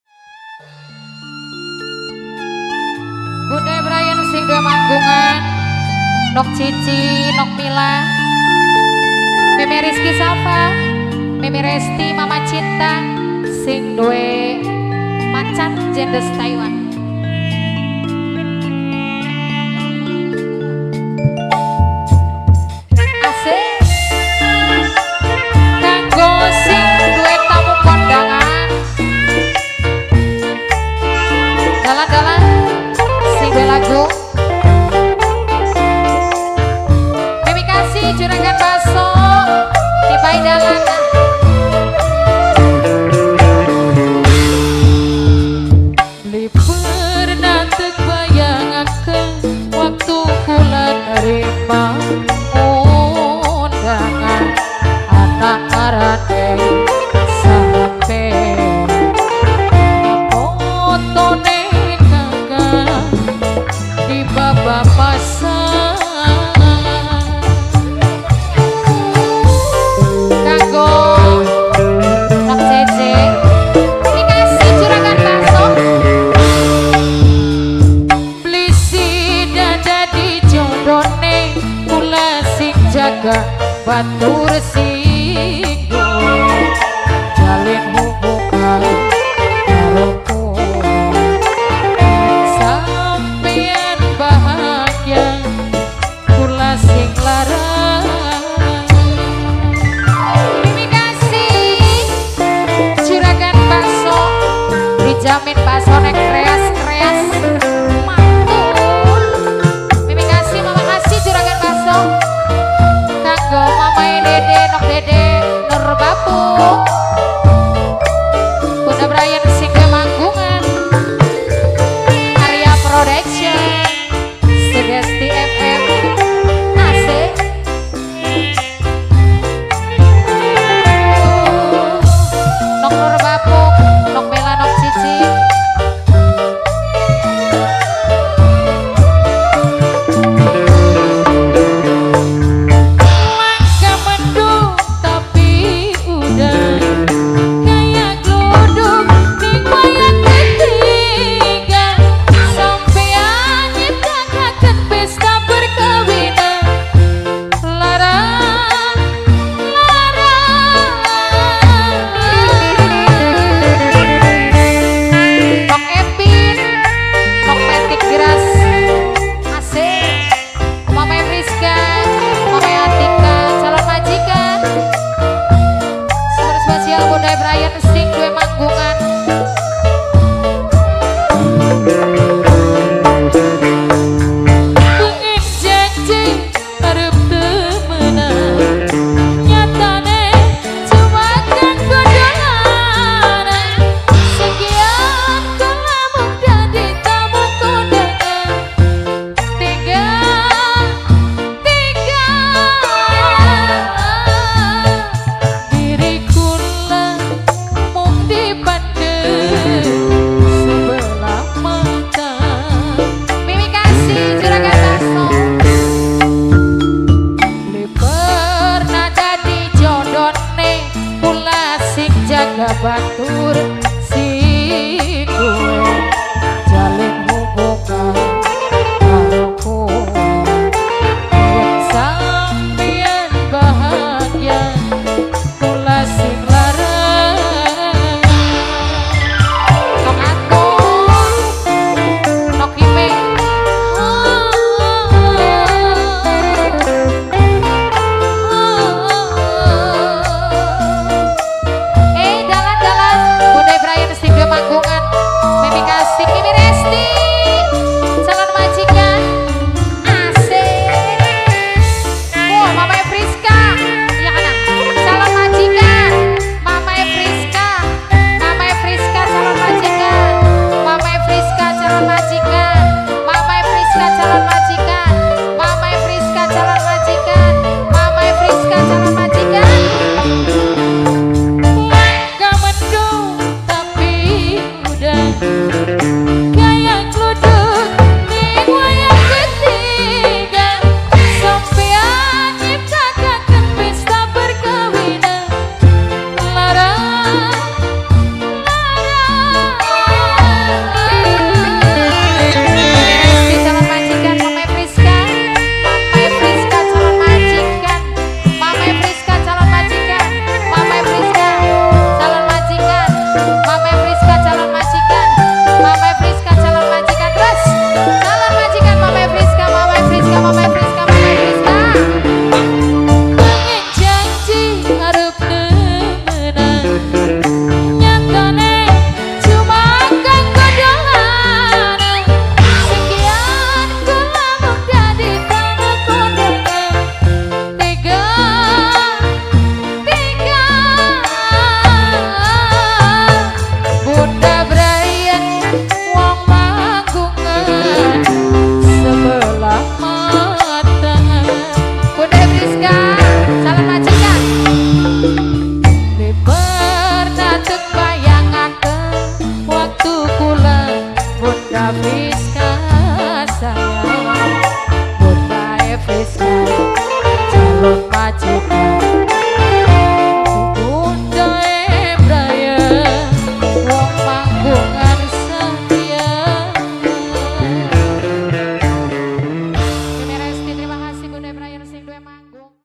🔴 TAMU KONDANGAN || SATRIA MUDA || MINGGU 27 MARET 2022 - KOMBO - TERISI
Pesta Hajatnya : Kel. Bpk. Muammae,SE / Ibu. Susilawati
Pelaksanaan : Minggu 27 Maret 2022
Tempat : Blok Kombo 1 Belakang Kantor Kuwu Desa Jatimulya
Kec. Terisi - Indramayu